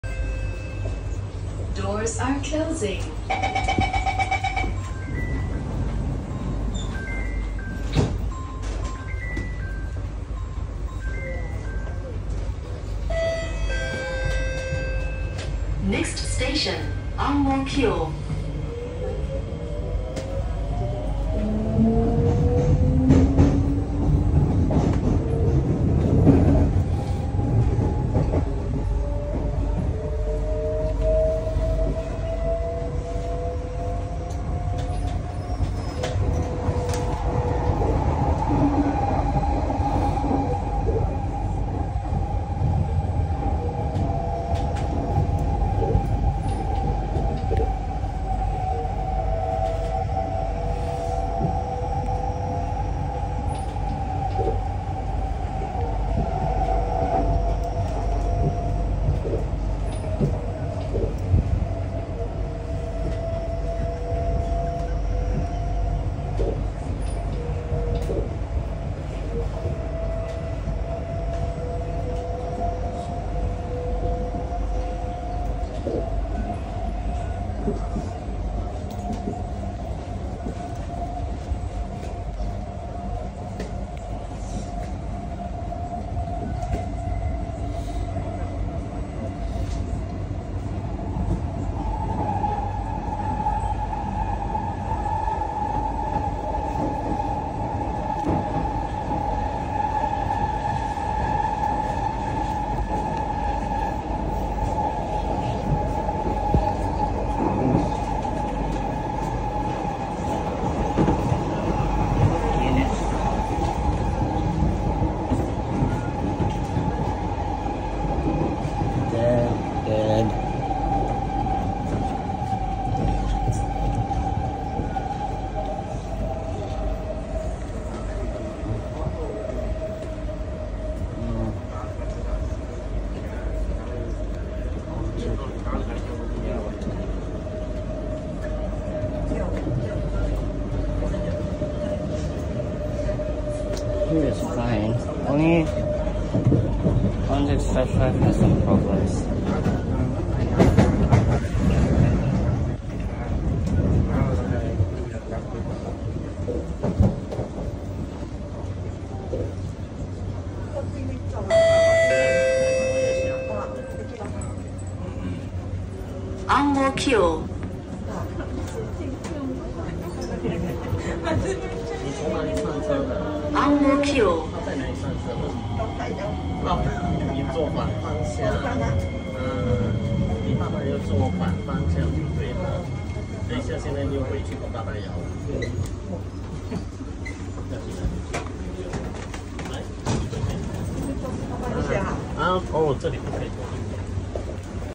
MRTravels on the North South Line: C151B Trainset 655/656 from Bishan to Ang Mo Kio
This video was filmed on 1 August 2023.

C151B 655/656 NSL TRN 208 FD LN